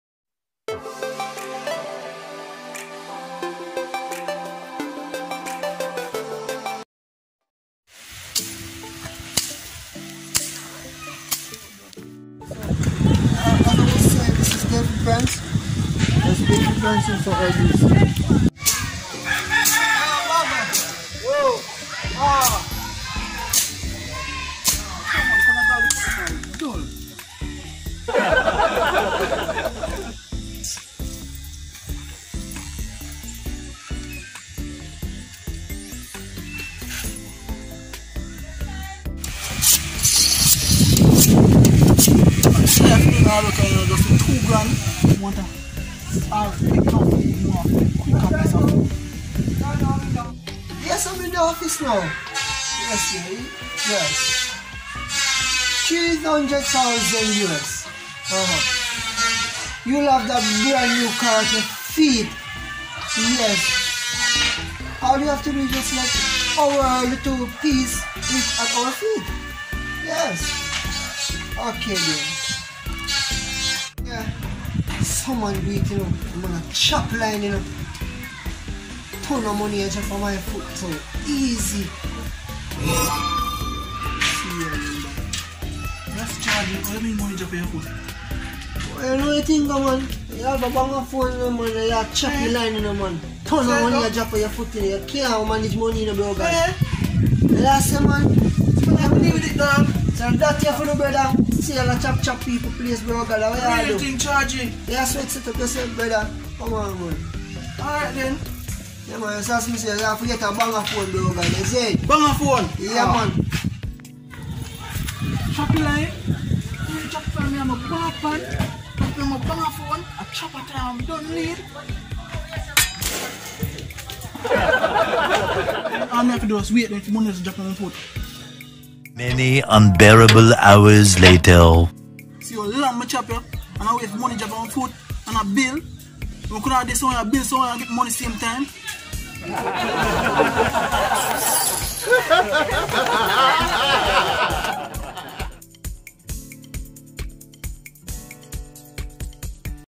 Choppa line🥴🥴🥴💰
A man overheard a guy on his phone about how to make easy money..he was informed on how to acquire this money but misinterpreted what the man said. stay tune to see what he did instead..